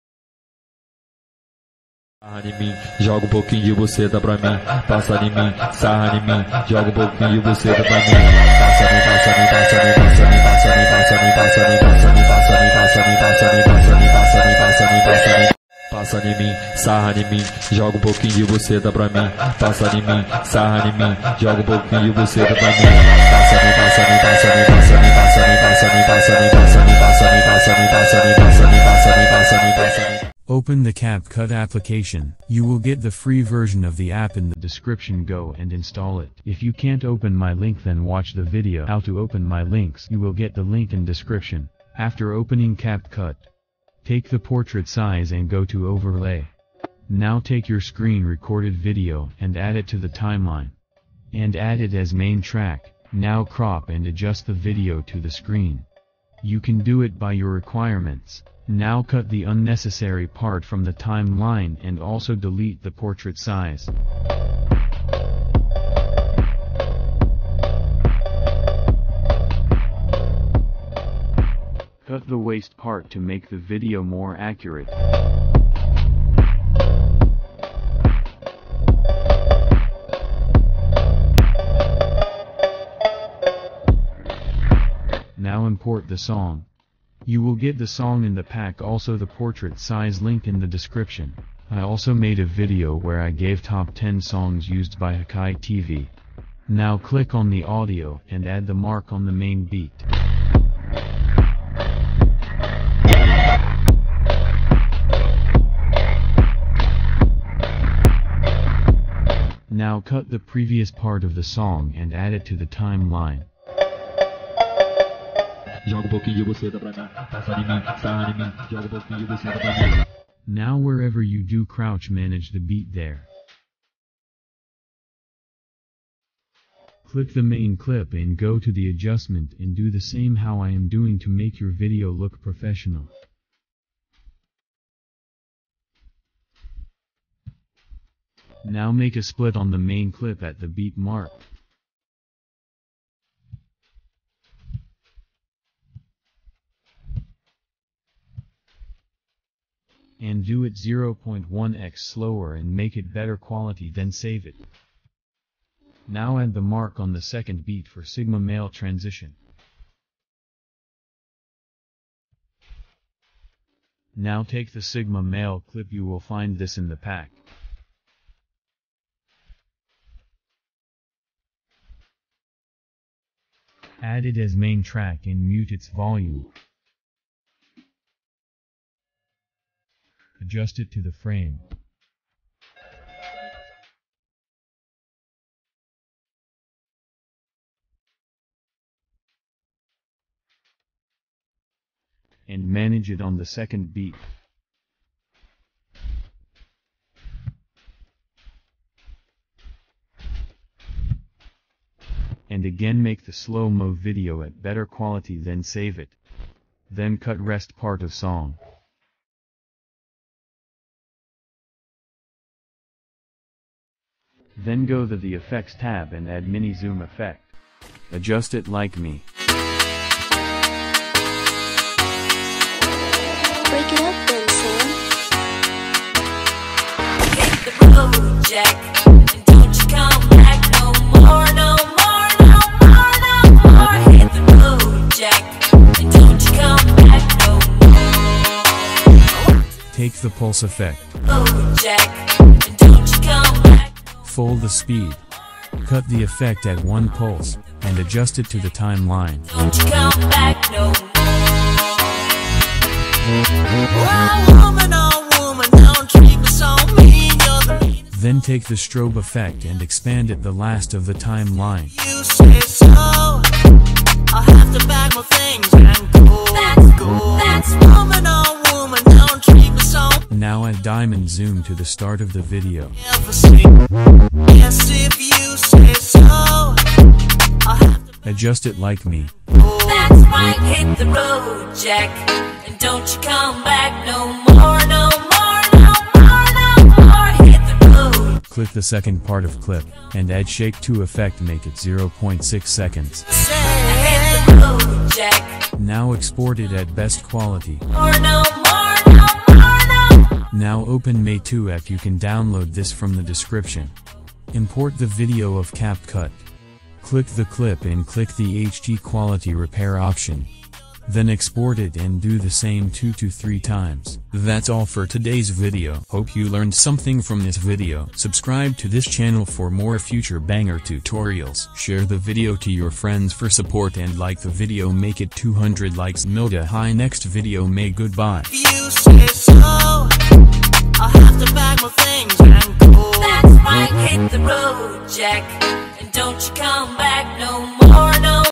Hakai Tv Viral Sigma Male 🗿 Short Editing Tutorial In Android | Hakai Tv 🔥Editing Secrets Revealed
To Get These you must have to download the Pack which link is given below, And you can also download font packs and all required Apps Like Kinemaster, Ps Touch, Pixellab and picsart.

████████Apps Required████████

███████Pack Download████████

👉How To Add Fonts In Picsart?